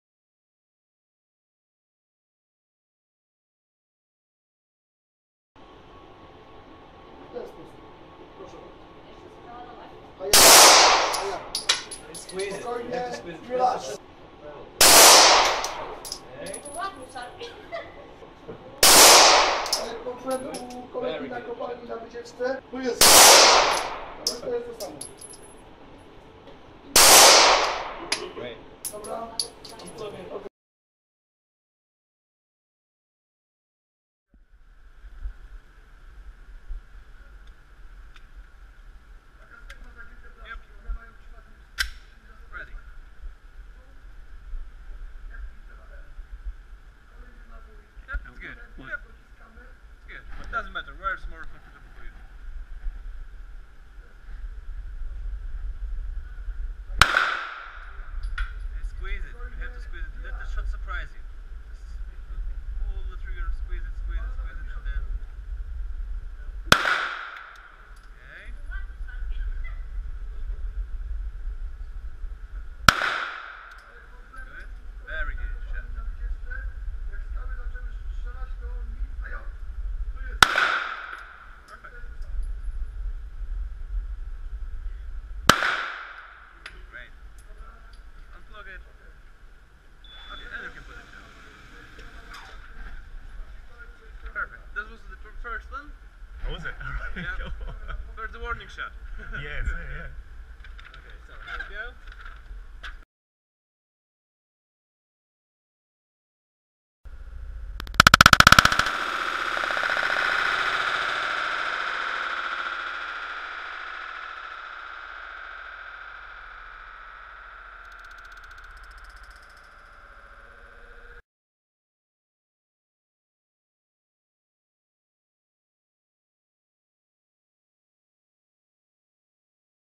Glock 17
Firing a Glock 17. (9mm).